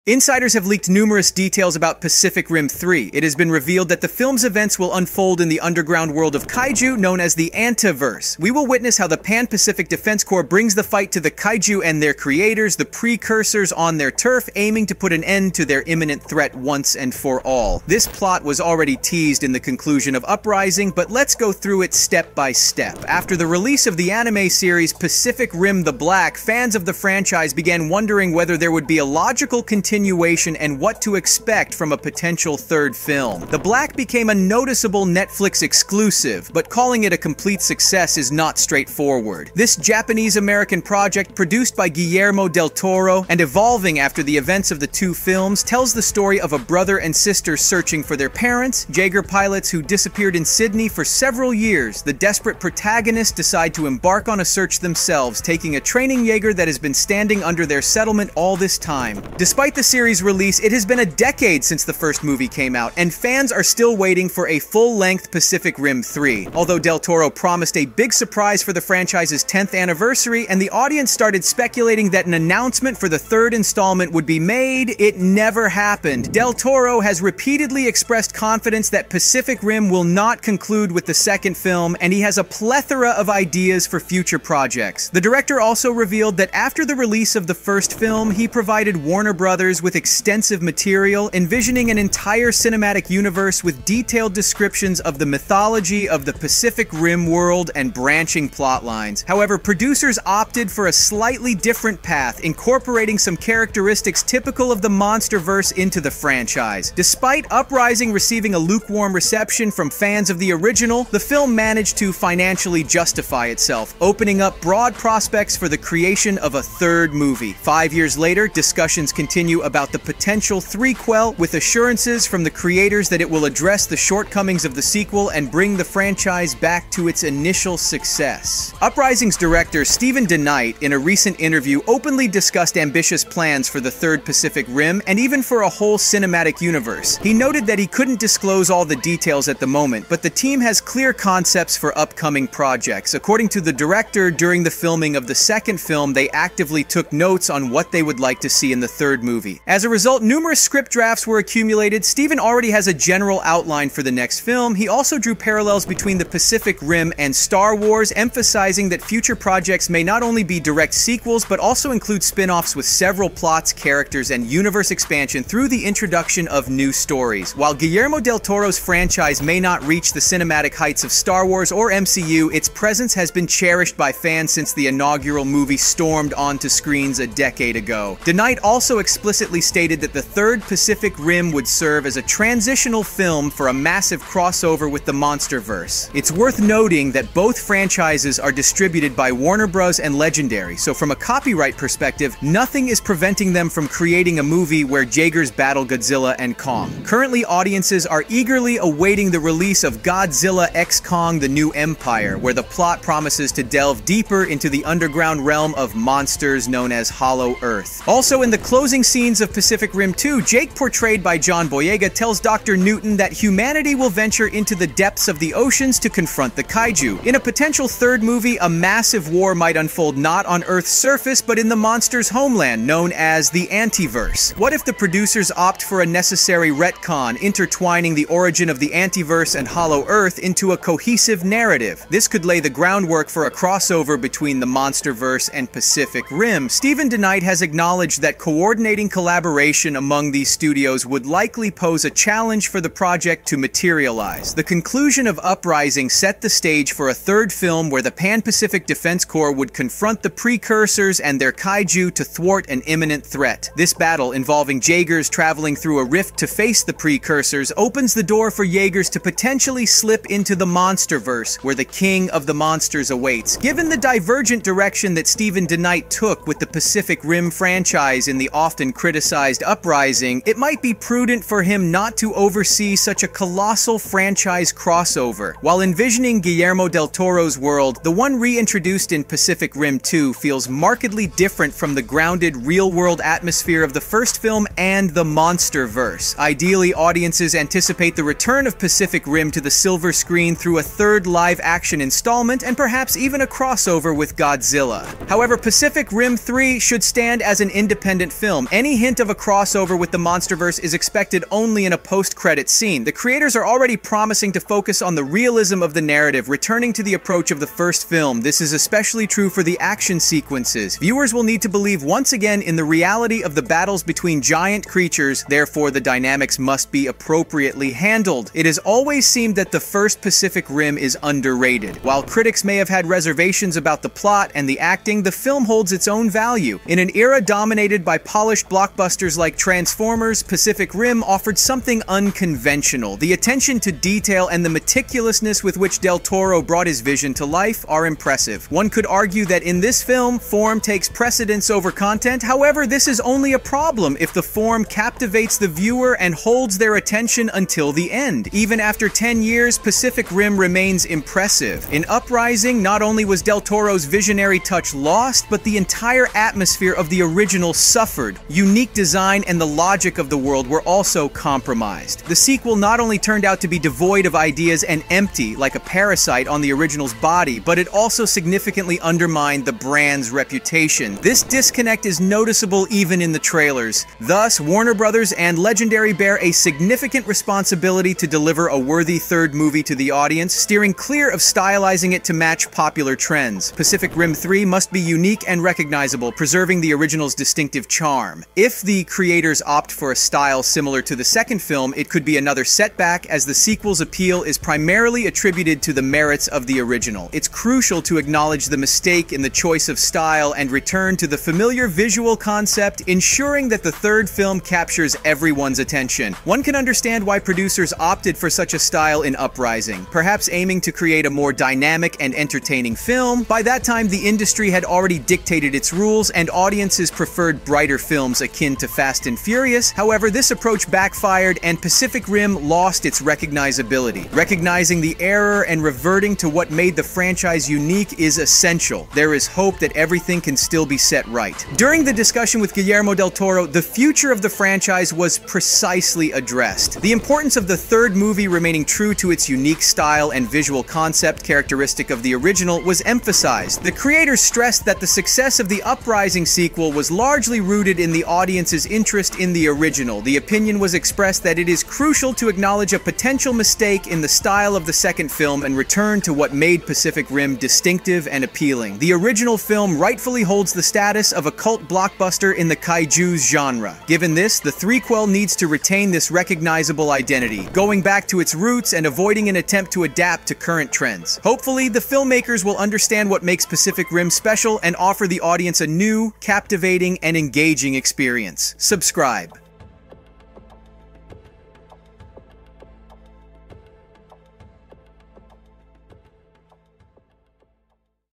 Pacific Rim 3: Cataclysm (2025) Warner Bros. Movie
At the end of the Pacific Rim: Uprising, we were teased about the plot of the Pacific Rim 3, which could unfold in the Anteverse. We're likely to see a global war with kaiju, and Guillermo Del Toro also hinted at a potential crossover with Godzilla, Kong, and the whole Monsterverse.

00:00 Future Plans
02:58 MonsterVerse & War
04:55 Main Plot
07:11 Del Toro’s Vision